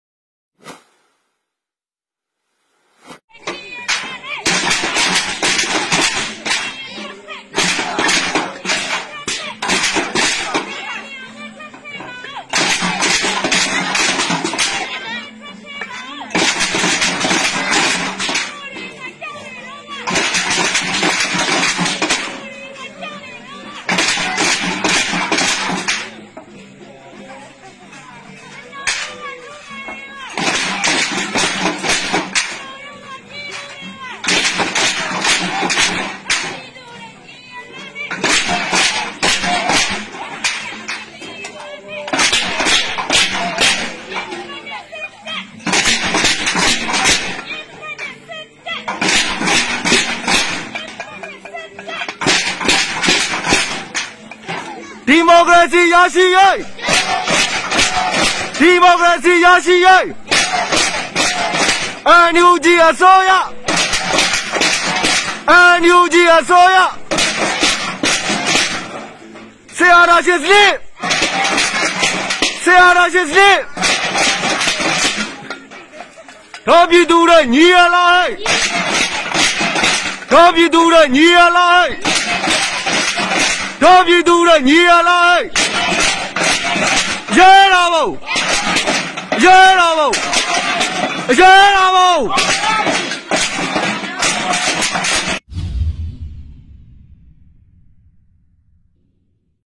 ရေစကြိုမြို့နယ်က သံပုံးတီး ညသပိတ်
မကွေးတိုင်းဒေသကြီး၊ရေစကြိုမြို့နယ်က ကျေးရွာတစ်ရွာမှာ ဇူလိုင် ၂၄ရက်၊ညပိုင်းက စစ်အာဏာရှင်ပြုတ်ကျရေး သံပုံးတီးညသပိတ်ပြုလုပ်ခဲ့ကြပါတယ်။